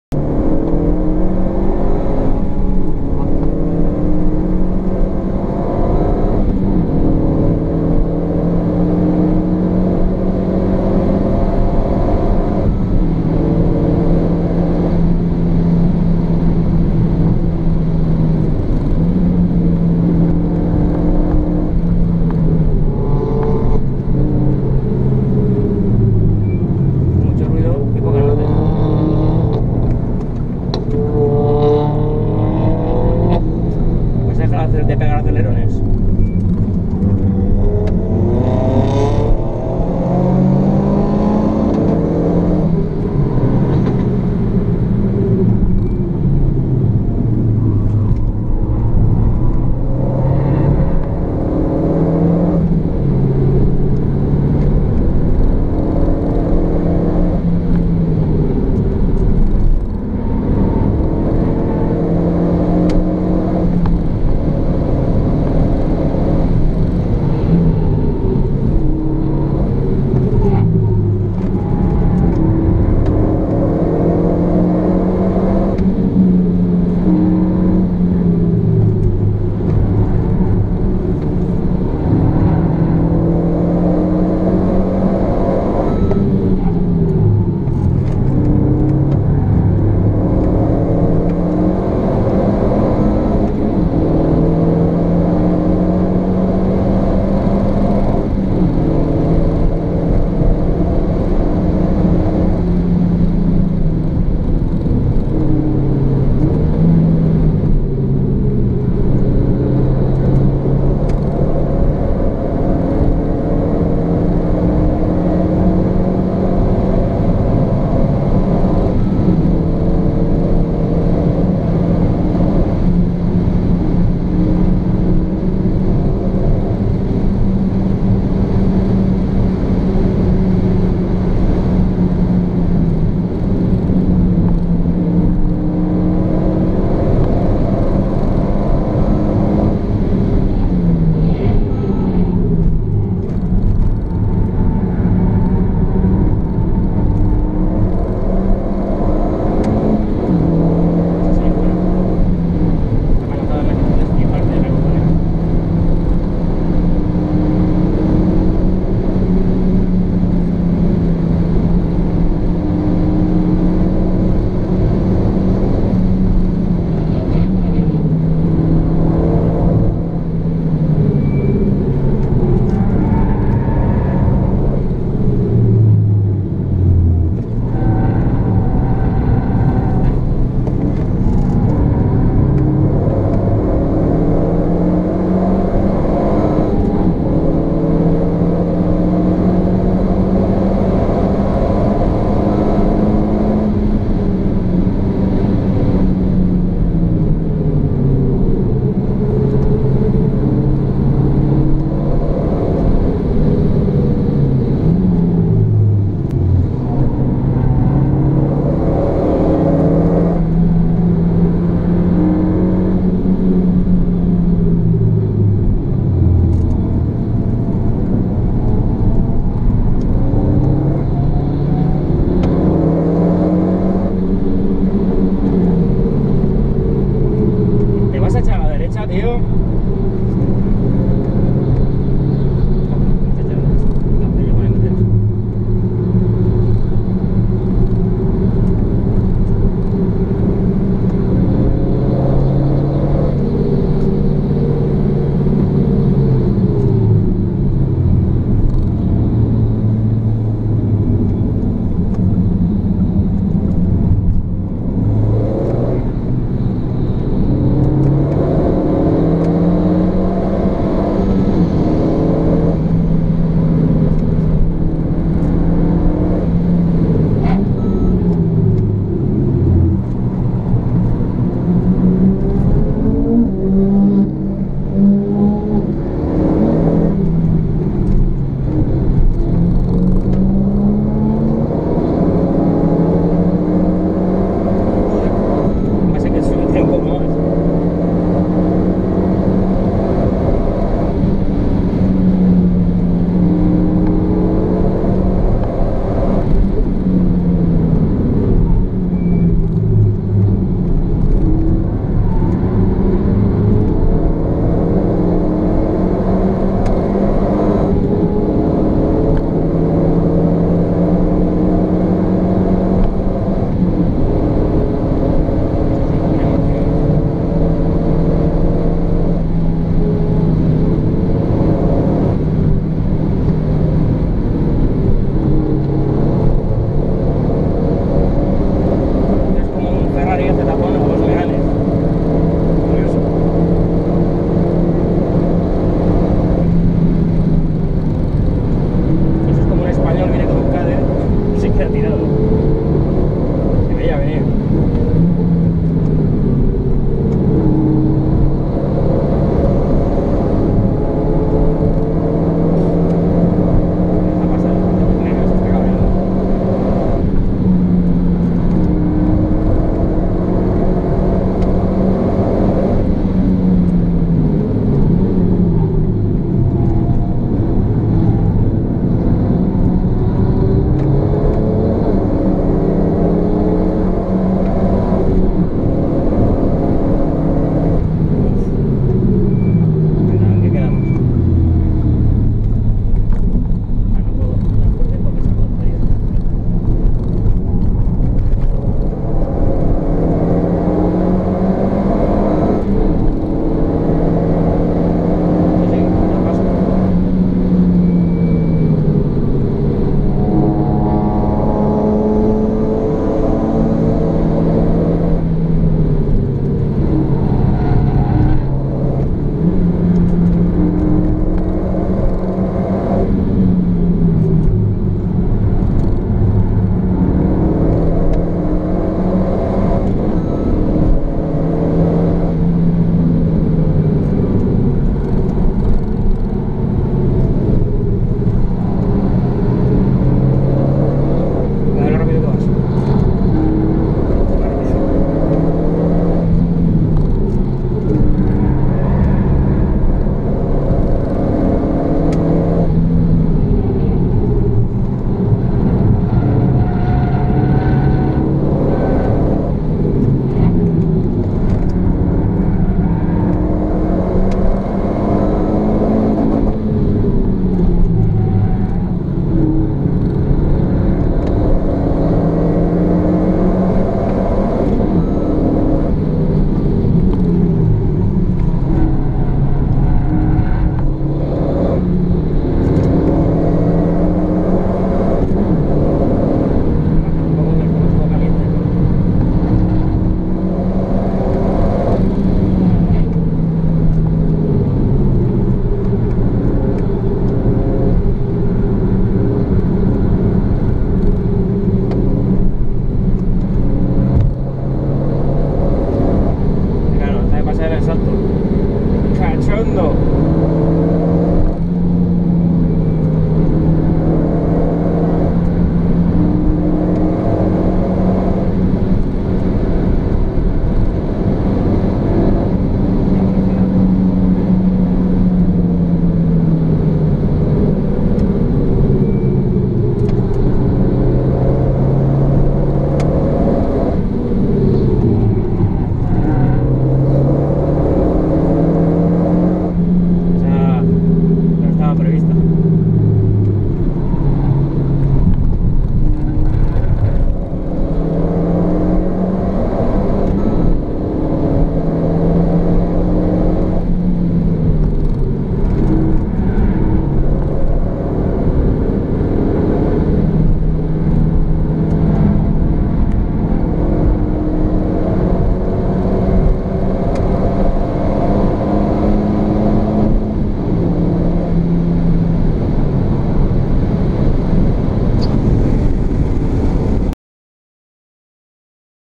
Fun lap in the Nürburgring Nordschleife BTG - Megane RS250
Vuelta al anillo norte (Nordschleife) de Nürburgring. Bastante tráfico, unos 9 minutos. Pero con momentos divertidos entre tanto coche al que tanto le cuesta echarse a la derecha.

Siendo de la primeras vueltas REALES en el ring, bastante orgulloso, sobretodo del comportamiento del Megane.

Ruedas Nankang NSR2.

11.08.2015

ENG: one of my first laps at the Nordschleife, getting a lot of fun !!